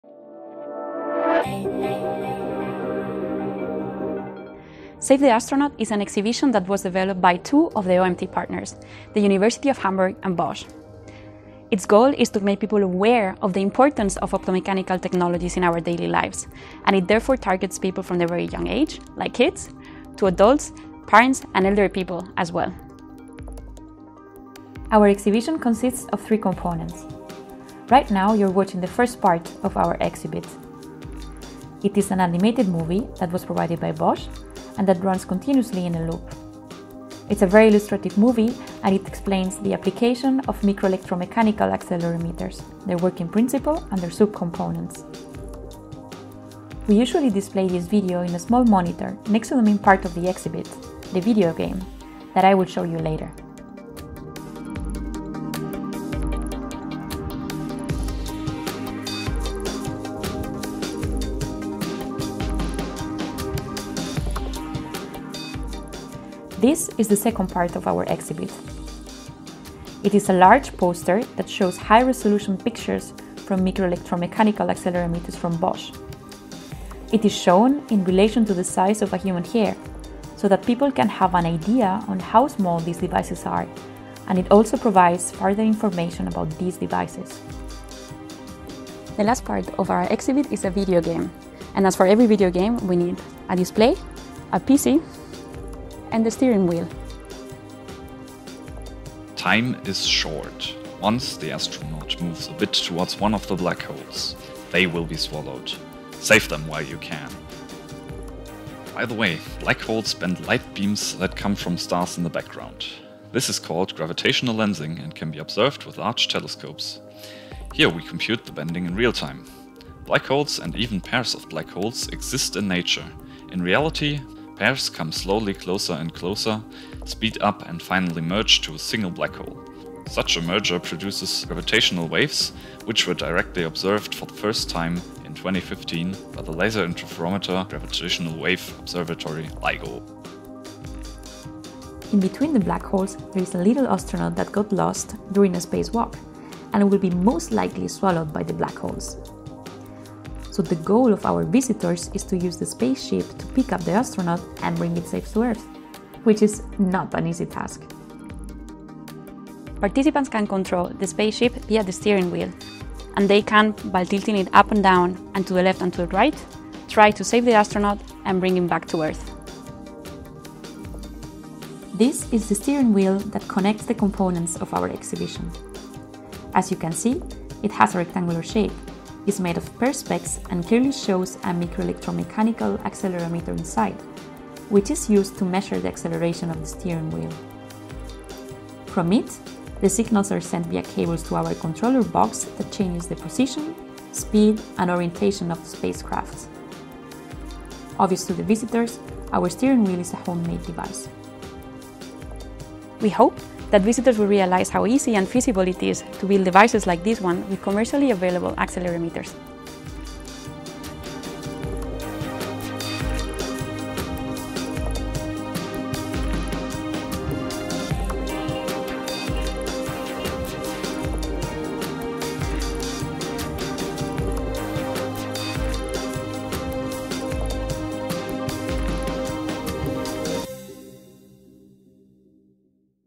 OMT saves the astronaut
Irene Sánchez Arribas, an OMT fellow from the University of Konstanz (Germany) describes OMT’s Traveling Exhibition. It was developed within the framework of the Marie Curie Skłodowska European Training Network (ETN), Optomechanical Technologies.  Save the Astronaut is an interactive video game that incorporates micro-electromechanical accelerometers which are used in everyday devices such as video game controllers, smartphones, and cars.  The exhibit traveled to different European cities within the ETN consortium where the ESRs presented the game at local events to demonstrate the importance and use of optomechanical technologies in our daily lives.